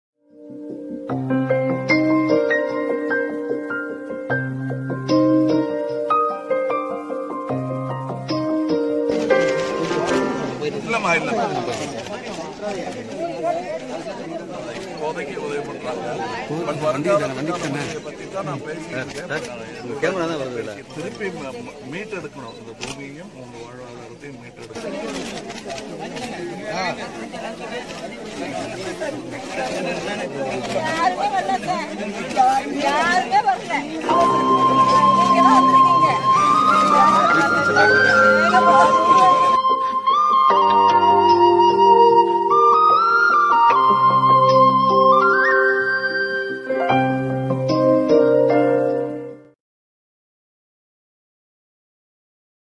கமல் ஹாசனைப் பார்த்ததும் கண்ணீர் விட்டு கதறி அழுத கஜா பாதிக்கப்பட்ட மக்கள்!
கஜா புயலால் பாதிக்கப்பட்ட பகுதிகளை நடிகரும், மக்கள் நீதி மய்யம் கட்சியின் தலைவருமான கமல் ஹாசன் நேரில் சென்று பார்வையிட்டுள்ளார்.